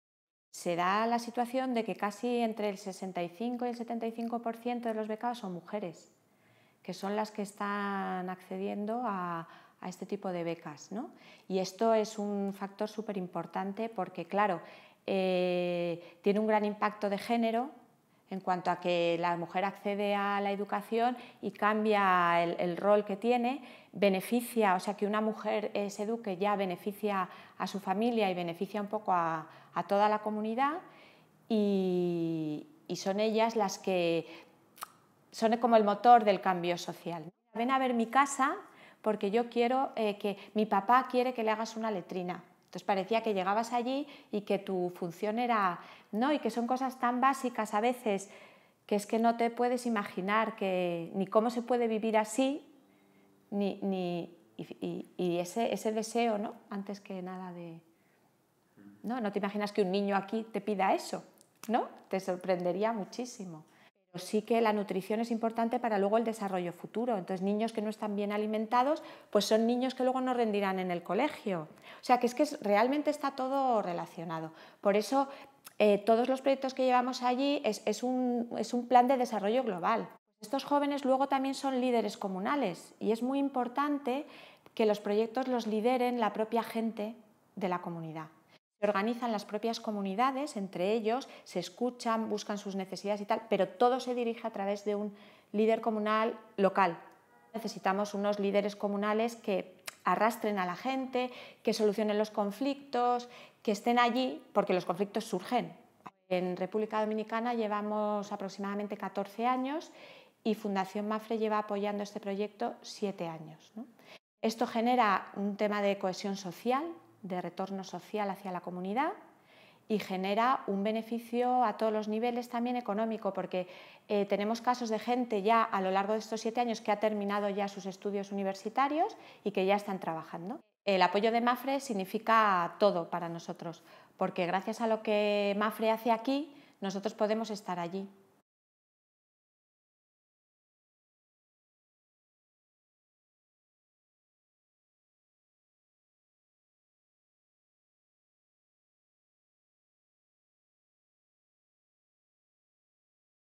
Proyectos sociales internacionales: sobre el terreno con Beatriz Mendoza
"Que una mujer se eduque beneficia a su familia y beneficia a toda la sociedad"
Beatriz Mendoza, responsable de sensibilización de Nuevos Caminos, explica cómo los proyectos en República Dominicana se constituyen como un plan de desarrollo global que generan beneficio a todos los niveles.